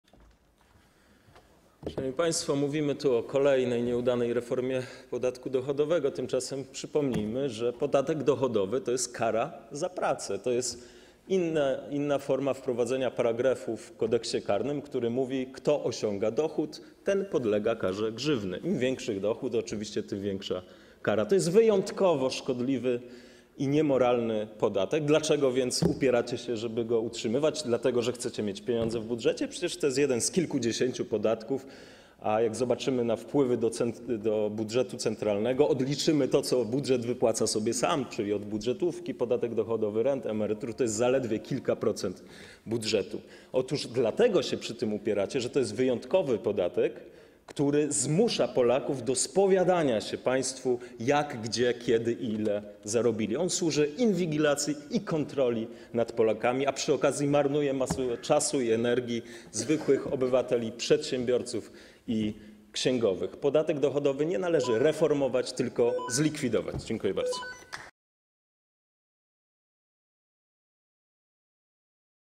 Konrad Berkowicz - o zatrudnianiu osób niepełnosprawnych [27 stycznia 2022 r.]
Popierasz? Udostępnij!

Sprawozdanie Komisji o rządowym projekcie ustawy o zmianie ustawy o zmianie ustawy o podatku dochodowym od osób fizycznych oraz ustawy o rehabilitacji zawodowej i społecznej oraz zatrudnianiu osób niepełnosprawnych